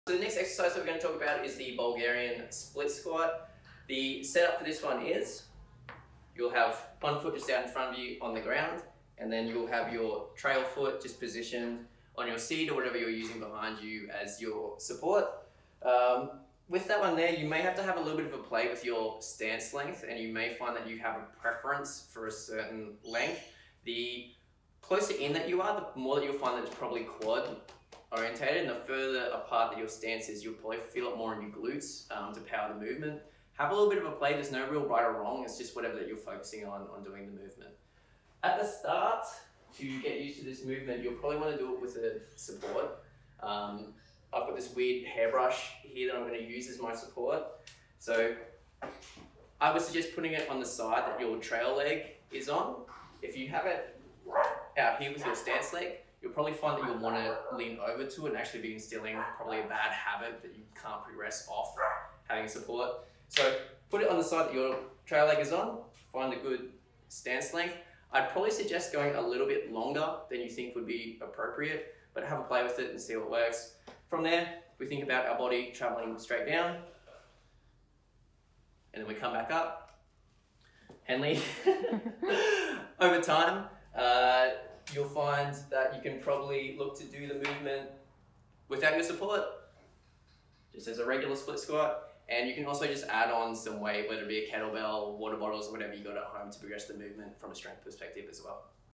Atkins Health - Julian Split Squat
Part of our exercise technique series, watch Julian from the Atkins Health Squad doing a Split Squat Exercise. Try it out and let us know how you go!